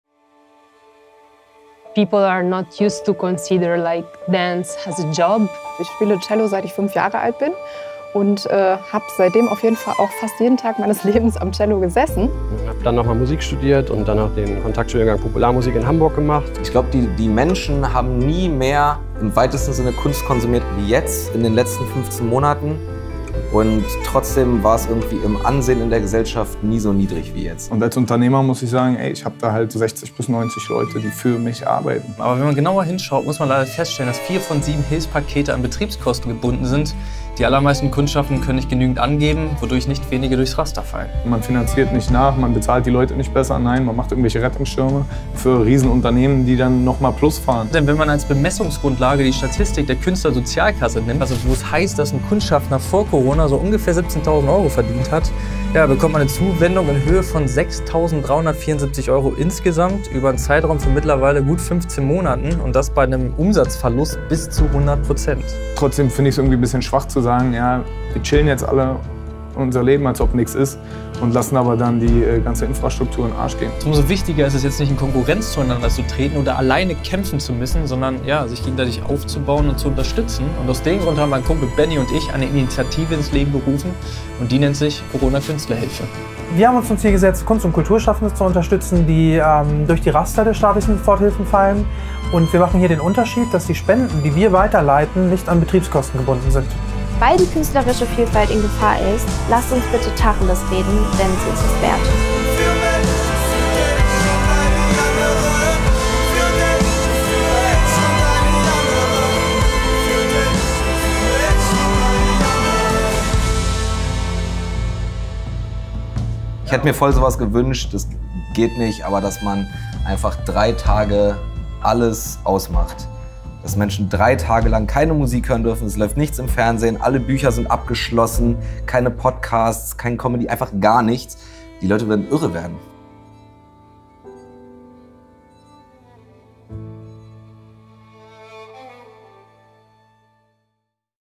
TRAILER: Warum die Kunst- und Kulturbranche am Ende ist
Schon mehr als 15 Monate ist die Situation in der Kunst- und Kulturbranche prekär. Viele Freiberufler*innen sind durch die Raster der staatlichen Hilfen gefallen.

Die Kultur- und Kreativbranche ist- vielleicht stärker als alle anderen Branchen- durch die notwendigen Beschränkungen in Zeiten der Pandemie bedroht. Sie war die erste Branche, die vom Shutdown betroffen war und wird die Letzte sein, die die Freiheit wieder zurückerlangt.
Mit über 1,8 Mio Erwerbstätigen hat die Kunst- und Kulturbranche mehr Beschäftigte als die Autoindustrie, die ca. 833.000 Menschen beschäftigt.

Darunter sind mehr als 258.000 Menschen freiberuflich tätig.

In den Sommermonaten kehrt nun wieder ein Hauch Normalität ein. Es wirkt als würde wieder alles seinen gewohnten Lauf nehmen. Doch die 15 Monate Berufseinschränkung hinterlassen ihre Spuren.

„Jeder möchte jetzt natürlich wieder auf Tour gehen, doch plötzlich ist die kleine Infrastruktur, die noch übrig geblieben ist so überlastet, dass es Ewigkeiten dauern wird, bis sie sich wieder aufbaut.“

Ein Zitat von Kontra K aus unseren Kurzfilm den ihr nun auf all unseren Social Media Kanälen anschauen könnt.

Lasst uns gemeinsam aufklären und lasstachelesreden denn die künstlerische Vielfalt ist in Gefahr! Helft uns indem ihr den Film verbreitet 🗣

Danke an alle Beteiligten, die diese Produktion möglich gemacht haben!

DIE PRODUKTION WURDE UNTERSTÜTZT VON
Mehr BB-Entertainment, Cineamo, Musical Dome Köln, Audio Post Leipzig, Chaussee-Soundvision, No51 Café & Bar Berlin, KWB Stadtpalais, Monologwelt

FILMPRODUKTION
Regie, Kamera & Schnitt - Timm Markgraf
Zweite Kamera - Finn Jordan Richter & Eric Ellerbrock
Making-of (Postproduktion & Kamera) - Eric Ellerbrock & Conway Kadenbach
Organisation & Projekt Management - Rebekka Antonia Eitel, Fabrice Riese & André Kallenbach (Monologwelt)
Technischer Support - Dennis Markgraf
Typografie - Sebastian Weiß
Faktencheck - Lea Pollmanns, Celina Gobez, Rebekka Antonia Eitel

TONMISCHUNG
Kino - Reemt Allerding (Chaussee-Soundvision)
Web & TV - Sylvio Pretsch (Audio Post Leipzig)

FILM LOCATION
Musical Dome Köln
No51 Café & Bar
Manjas Zimmer

FILMMUSIK
Komposition & Produktion - Timm Markgraf
Songwriting & Performance - Finn Jordan Richter
Klavier - Roman Goly
Cello - Moritz Brümmer
Geige - Max Eisinger
Drums - Sören Schalk
Percussion - Finn Jordan Richter
Backing Vocals - Rebekka Antonia Eitel
Gitarre, Bass, Rhodes - Timm Markgraf

Mix - Thilo Zirr

TEAM MEHR-ENTERTAINMENT / MUSICAL DOME
Eventmanager - Maximilian Bludau
Technischer Leiter - Andreas Strohecker
PR & Marketing - Corinna Steffens-Dieckmann
PR Managerin - Anna Höly
Pförtner - Mohamed Fadili

TEAM TONTECHNIK KAUFMANN
Lichtdesign/Operator - Steven Goetz
Technische Licht-Assistenz - Lea Varoquier, Klemens Kreuder

TEAM 21DX GMBH (CORONA TESTSTATION)
Alexa Epke, Julian Wolf, Baraa Abuarra, Lena Wolf

BESETZUNG (IN REIHENFOLGE)
Interview Gast - Felix Lobrecht
Interview Gat - Kontra K
Tänzerin - Irene La Monaca
Cellistin - Ella Rohwer
Pianist - Roman Goly
Cellist - Moritz Brümmer
Geiger - Max Eisinger
Initiator CKH - Timm Markgraf
Gitarristin - Anna Stucky
Drummer - Sören Schalk
E-Gitarrist - Thilo Zirr
Bassist - Dominik Bornhorn
Sängerin - Rebekka Antonia Eitel
Initiator CKH - Benjamin Klein
Sänger - Finn Jordan Richter

VIELEN DANK
Michael Ahlers, Fabian Fornalski, Jörn Hintz, Rike Tödter, Michael Rückert, Fabian Müller, Joy Klehr, 1st Class Session - Artist Support e.V., SCALA Programmkino Lüneburg

Dies ist ein unabhängiges Initiativprojekt von
Benjamin Klein & Timm Markgraf